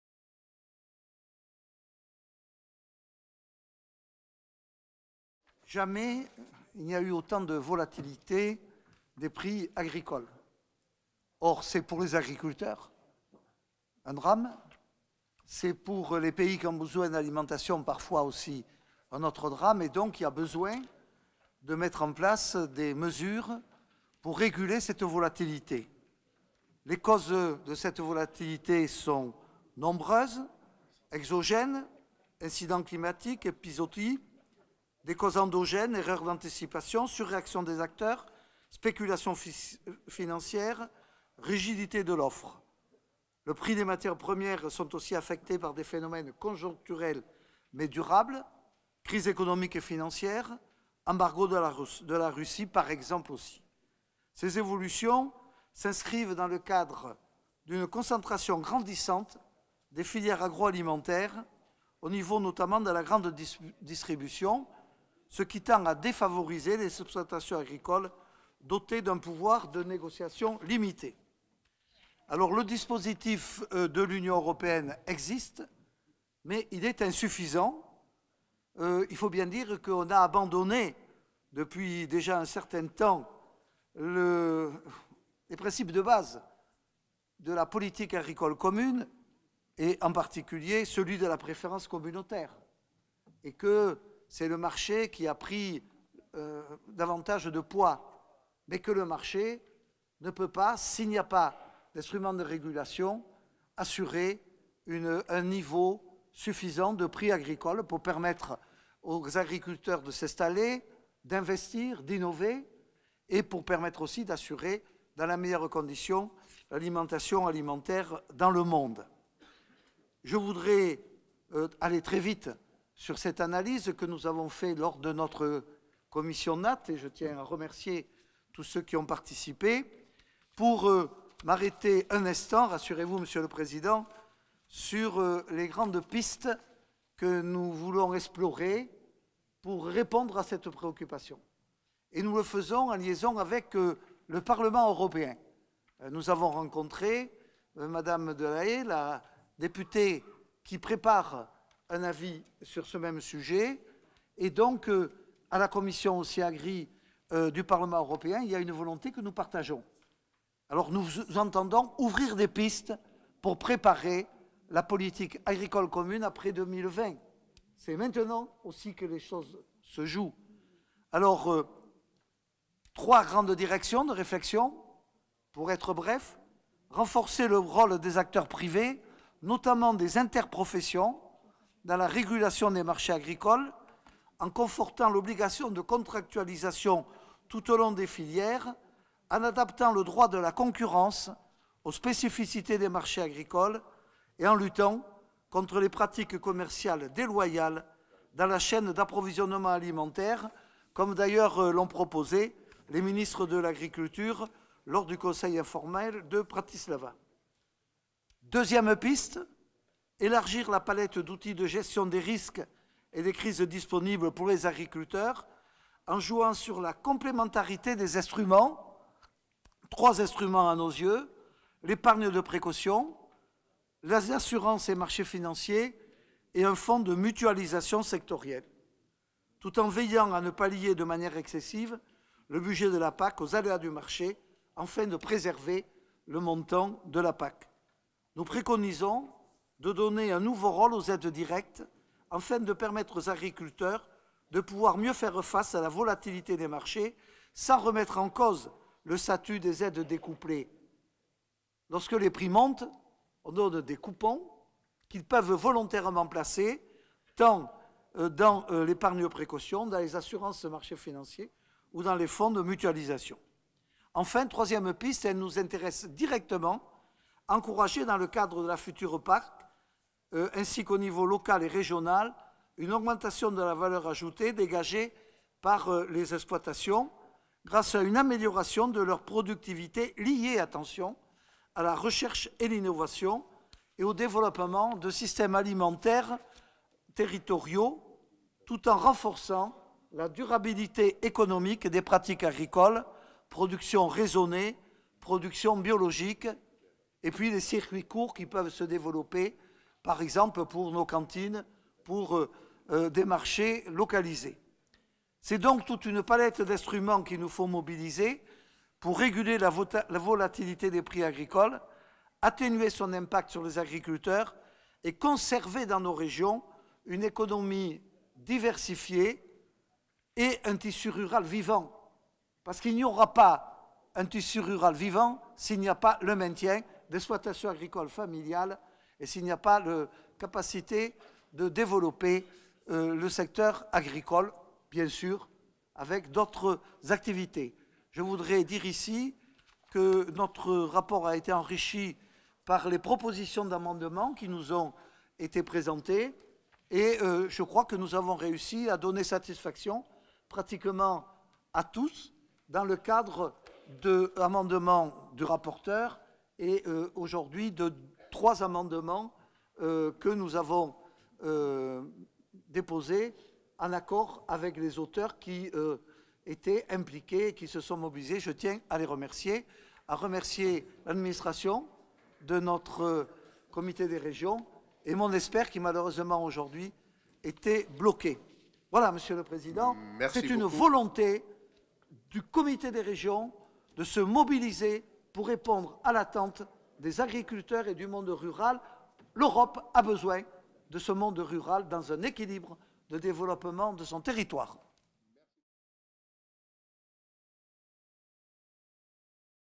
Jacques Blanc - 120th plenary session - European Committee of the Regions
Jacques Blanc, Member of the European Committee of the Regions and rapporteur of the opinion on Technical measures in fisheries, at the 120th plenary session of the European Committee of the Regions.